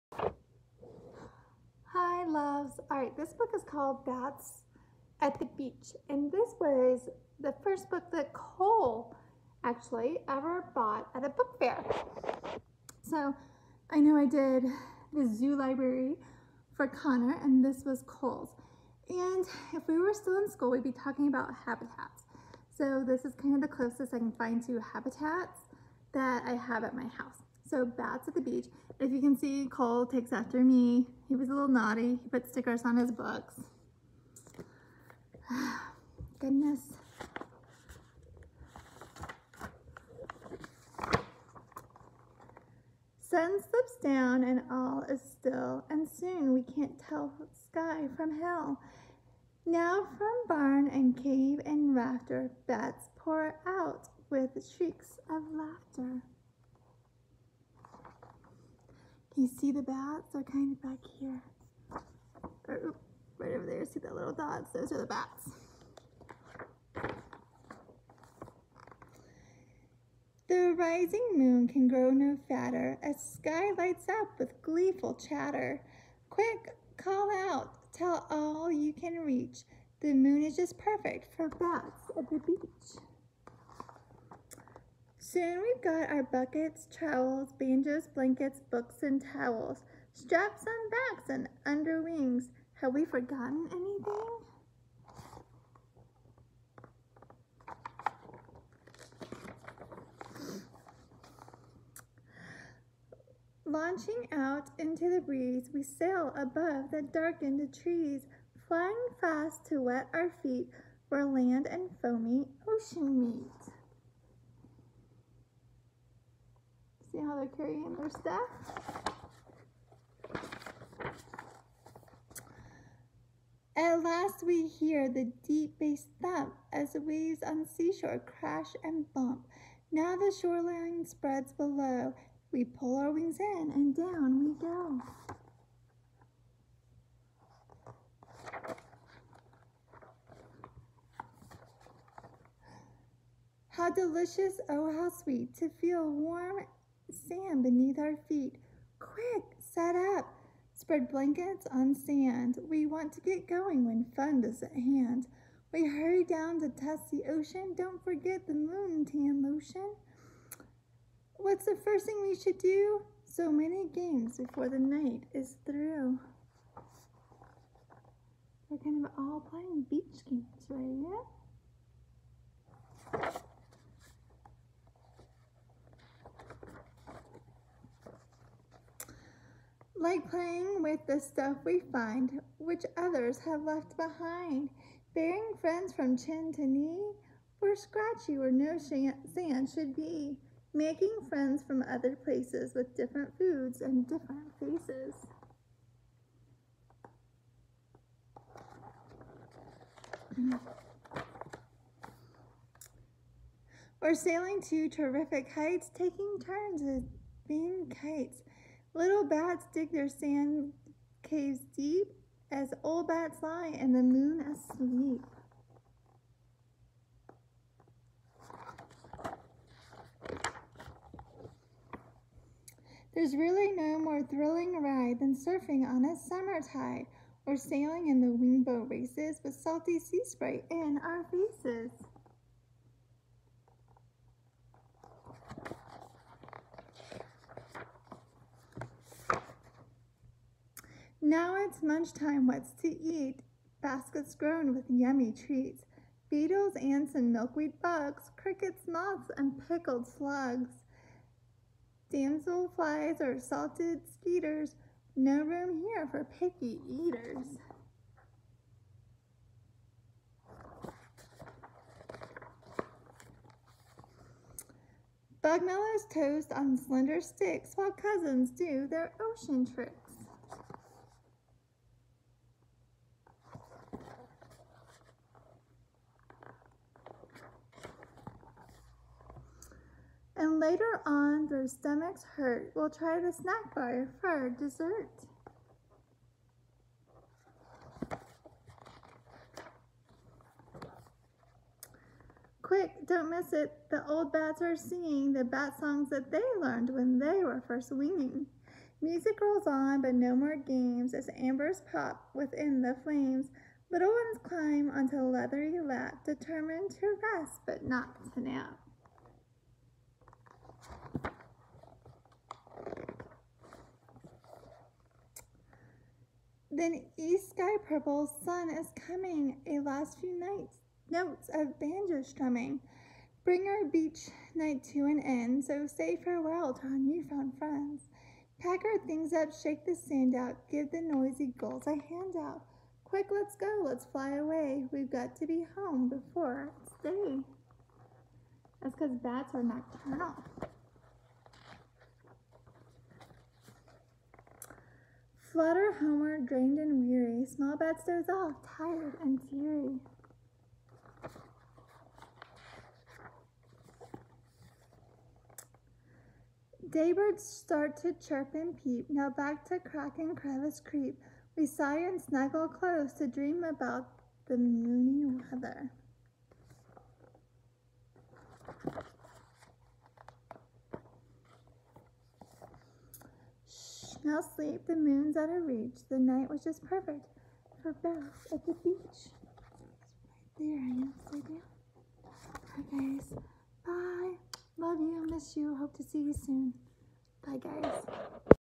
Bats at the Beach
Mrs. Haithcoat reads a story.